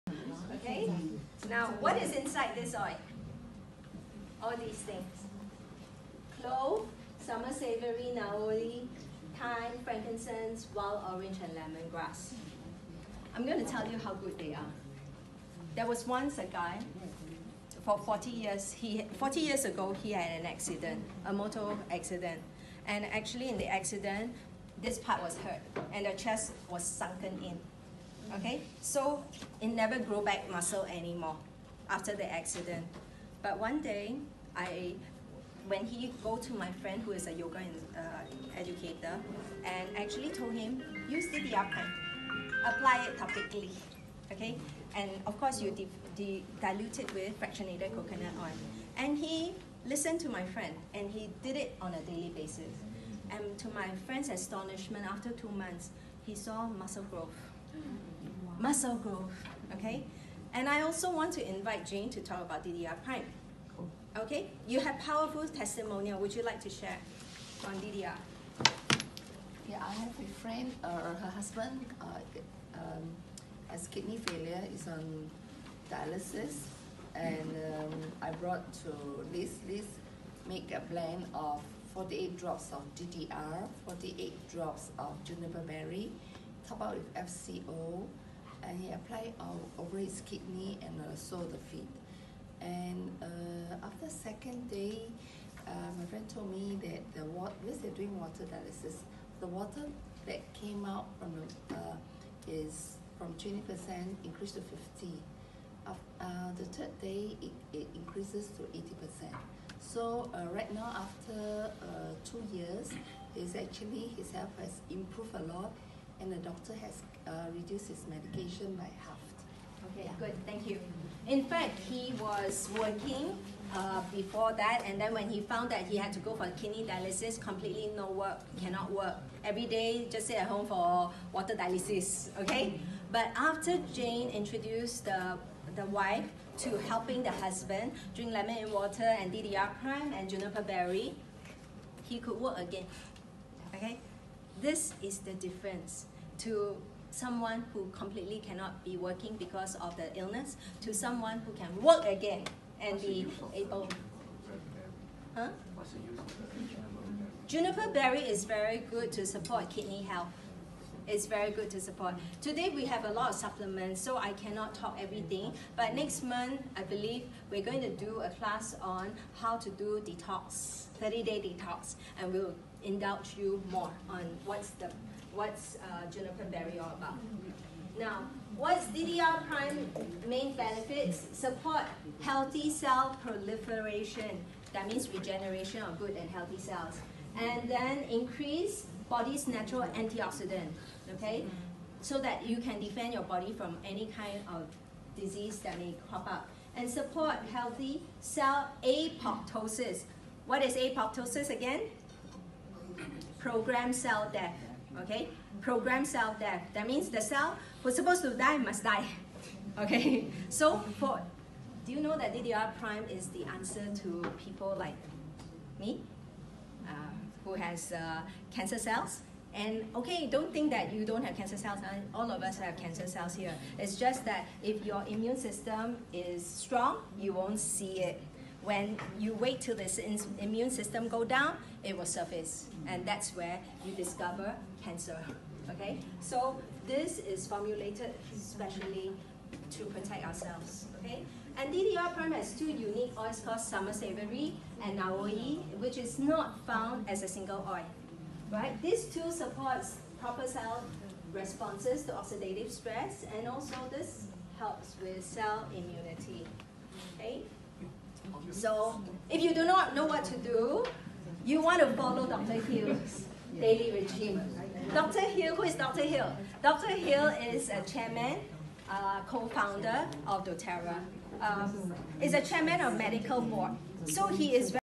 Elizabeth Ho shares about DDR Prime
DDR Prime Cellular Complex is an oil blend designed to support healthy cell functioning